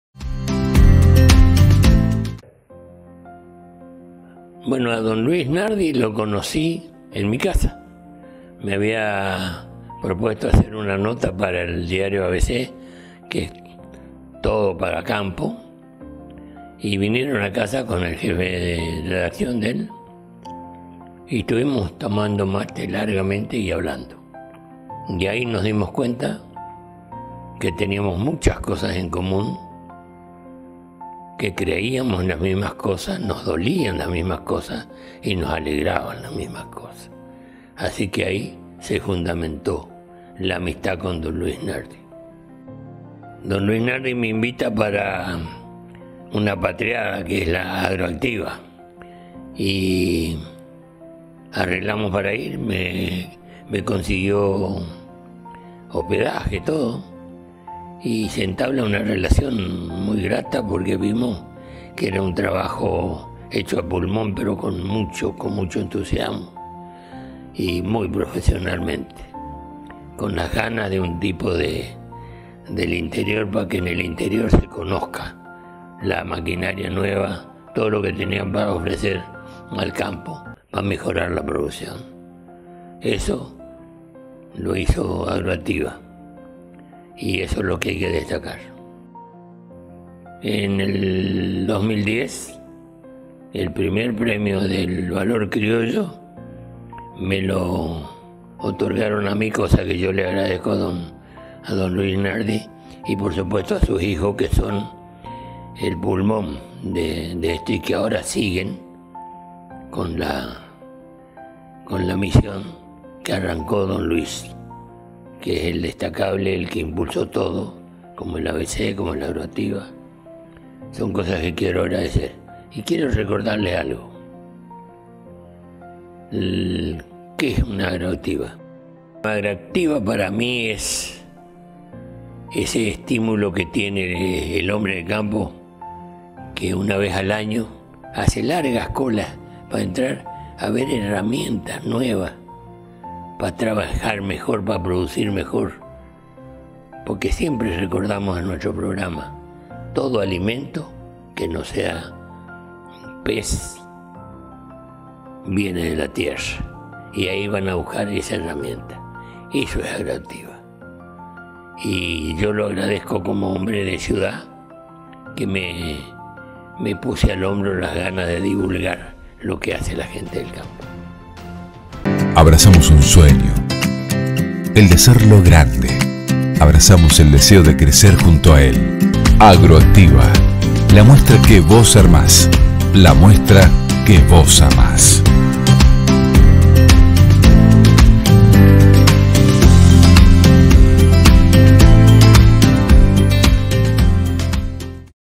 Don Luis Landriscina y su vínculo con Luis Nardi y AgroActiva
Don Luis Landriscina, es el mejor cuentista argentino por su estilo narrativo y su humor basado en los usos y costumbres regionales del país y el Río de la Plata. Amigo de AgroActiva, nos cuenta cómo se conoció con Luis Nardi y qué significa para él la mega muestra.